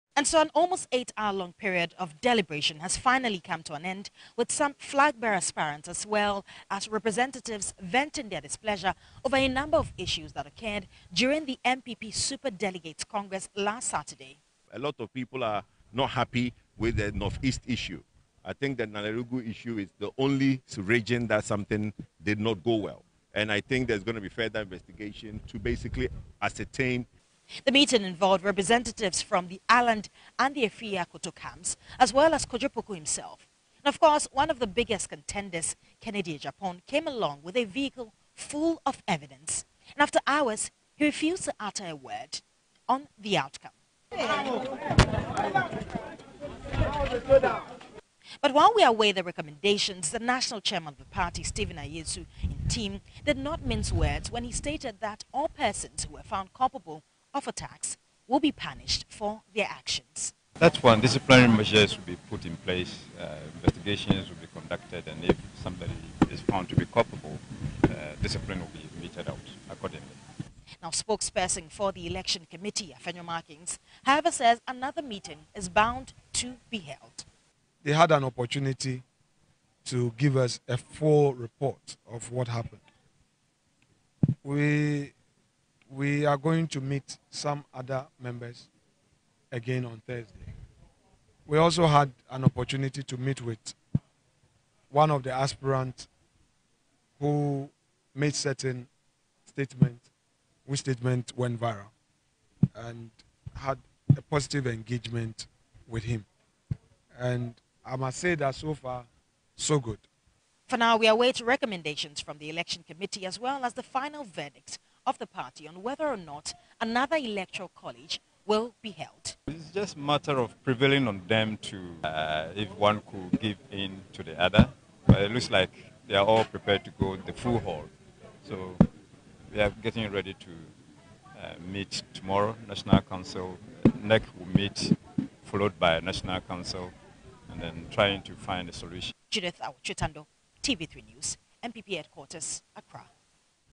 NPP Special Delegates conference: Election committee to investigate reports of assaults
The NPP says it will conduct a full-scale investigation into the alleged assault of some polling agents during the super delegates conference.

The election committee of the party held a marathon meeting with some candidates who contested the election.